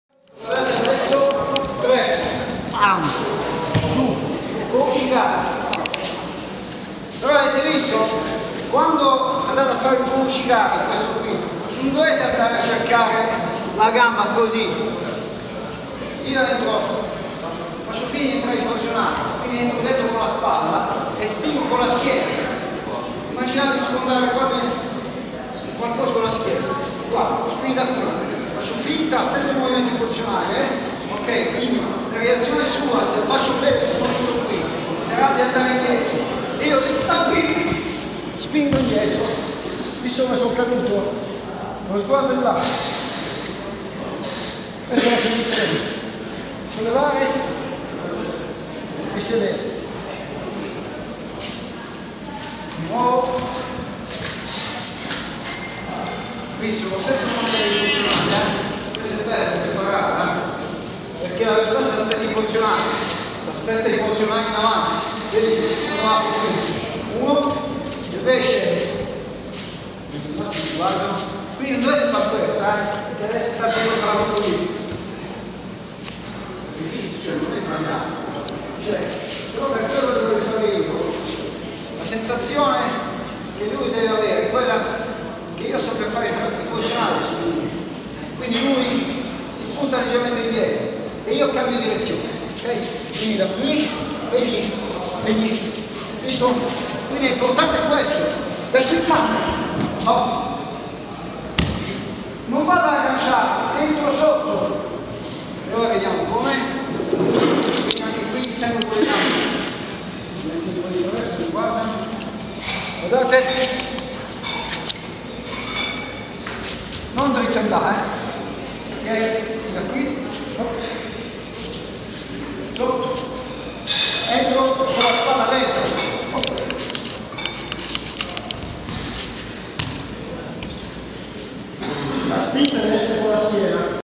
Judo Bollenti Spiriti 6 Marino Cattedra 4
Federazione Judo San Marino stage di judo 2017, Marino Cattedra